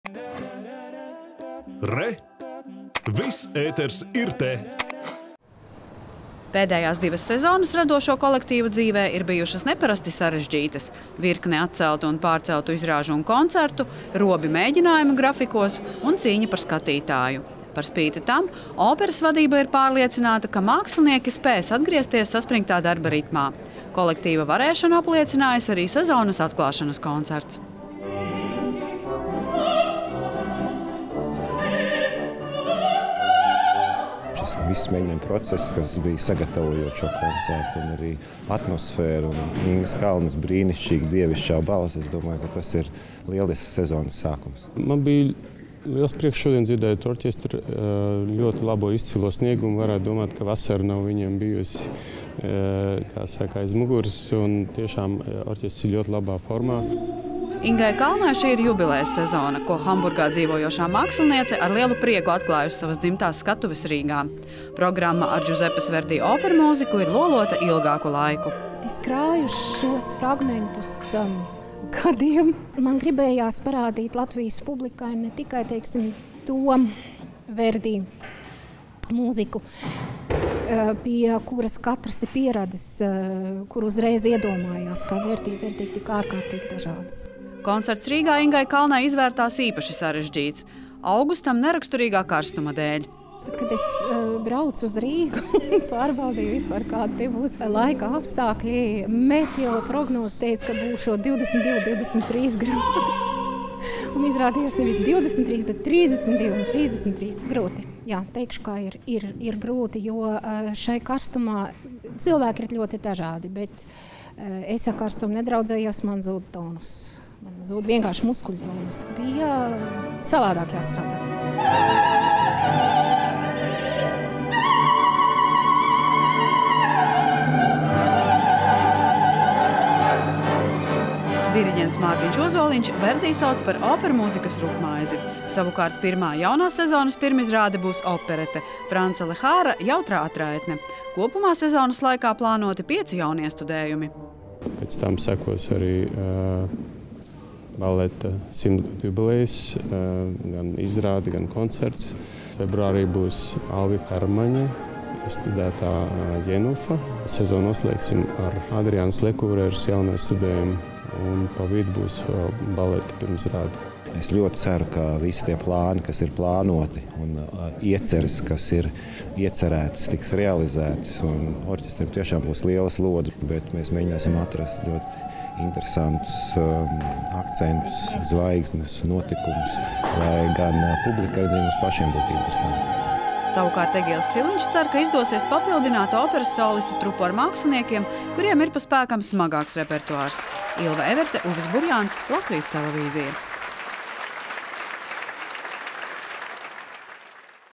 Ar Ingas Kalnas solokoncertu sāk operas un baleta sezonu
Jauno sezonu sāk arī Latvijas Nacionālā opera un balets. Vakar sezonas atklāšanas koncertā uz skatuves bija dziedātāja Inga Kalna un operas orķestris Mārtiņa Ozoliņa vadībā. Koncerta programmā – Džuzepes Verdi opermūzika.